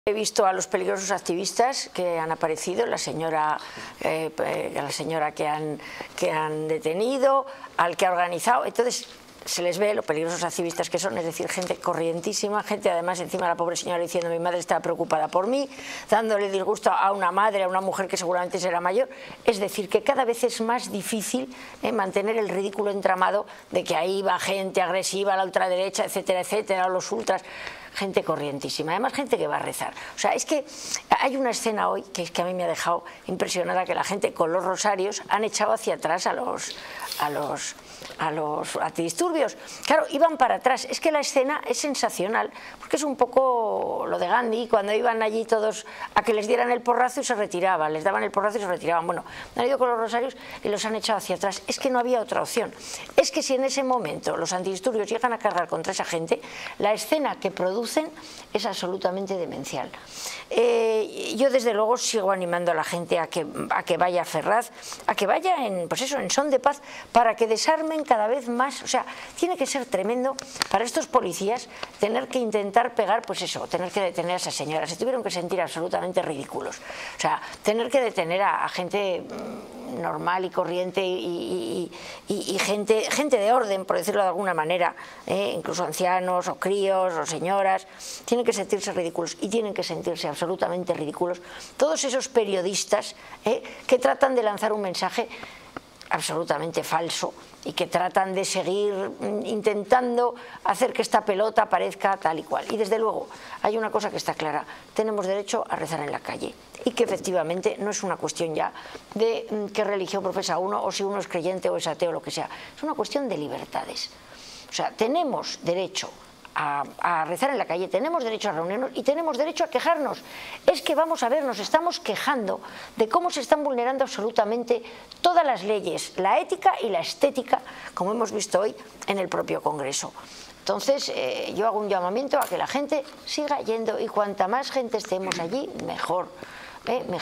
💥Alicia Rubio: La gente de Ferraz ha ido con el rosario y ha echado hacia atrás a la policía 💥
Queriendo volver este martes, la arrestada por la Policía de Marlaska por rezar el Rosario a pesar de la prohibición del delegado del Gobierno de Madrid, ha regresado a la Parroquia del Inmaculado Corazón de María, ubicada en la intersección con la calle Ferraz, donde se encuentra la sede del PSOE y Marqués de Urquijo, lugar de las manifestaciones en contra de las concesiones de Pedro Sánchez a los independentistas.